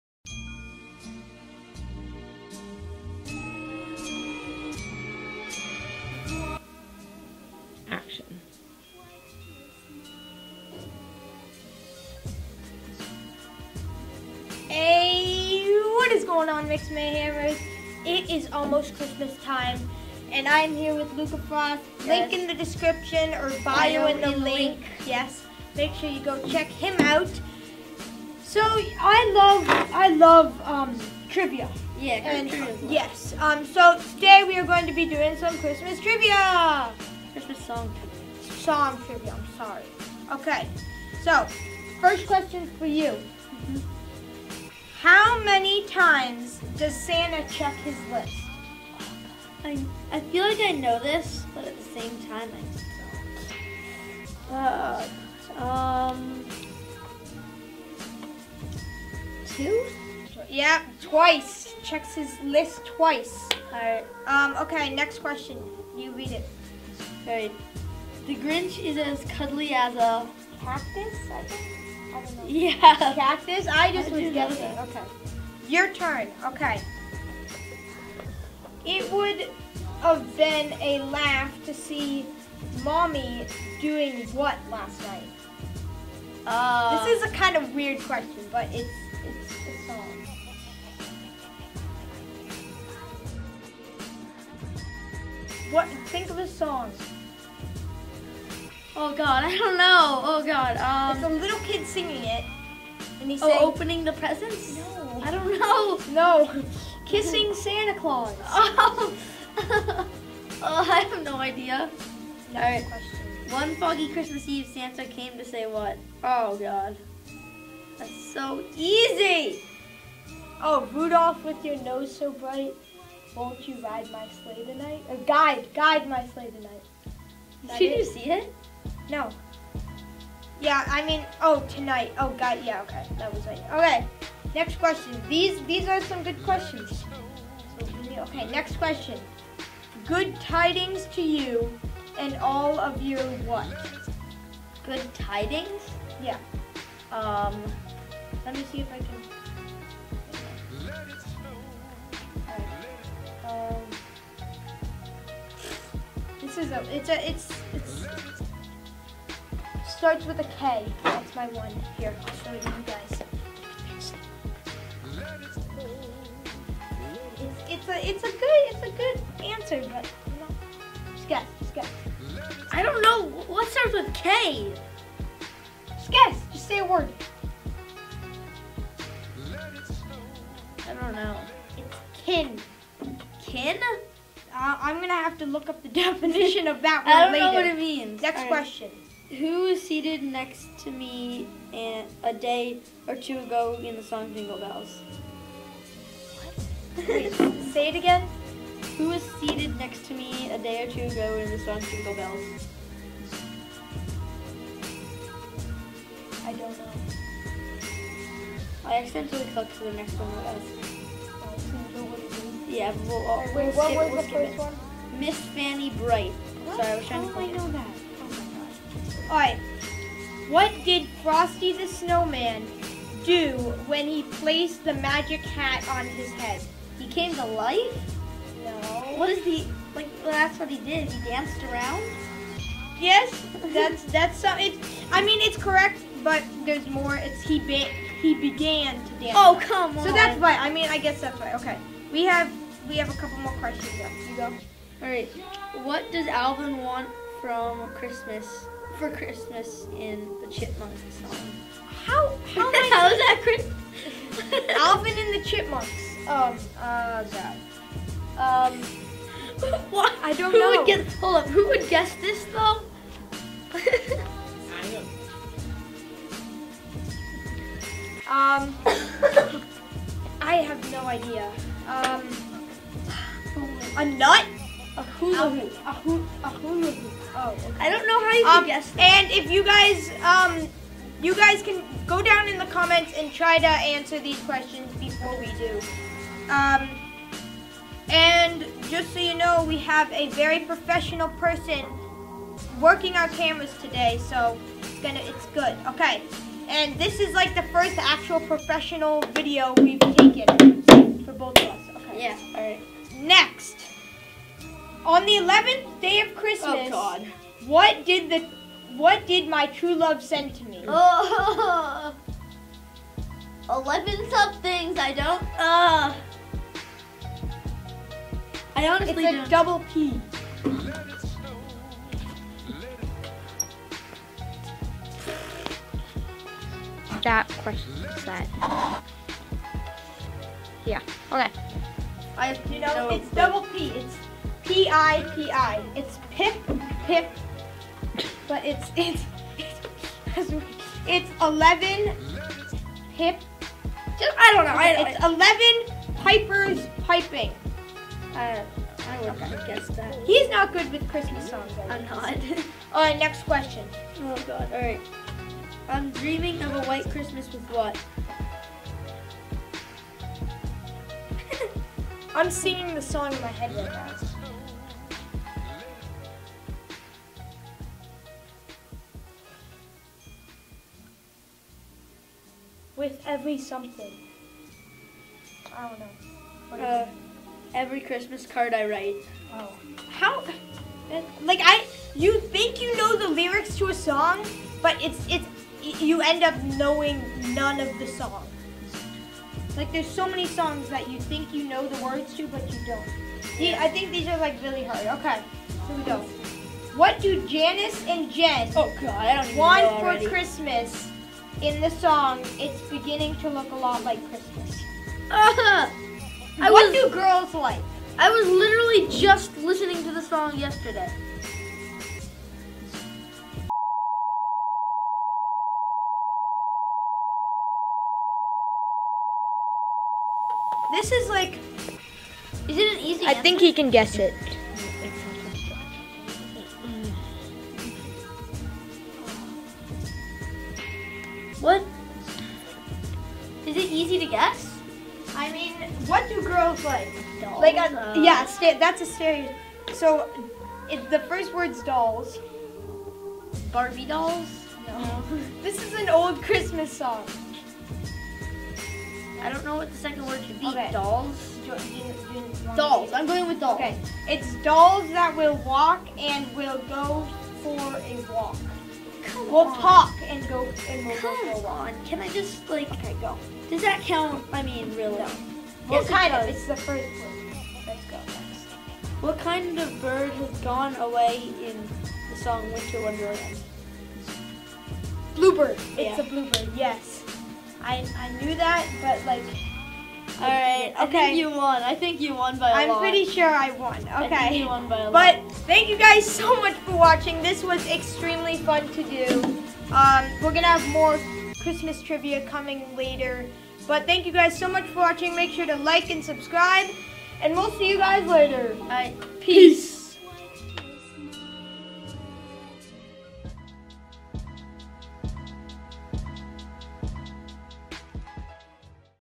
Makerspace Mondays: Christmas Trivia
Christmas Trivia, as the name implies, is a game show hosted by youth members at Somerville Media Center focusing on all things Christmas! Part of a compilation of the best clips from this years' Makerspace Mondays youth media showcase. Learn more about Makerspace Mondays and our other SMC Youth Programs by visiting today.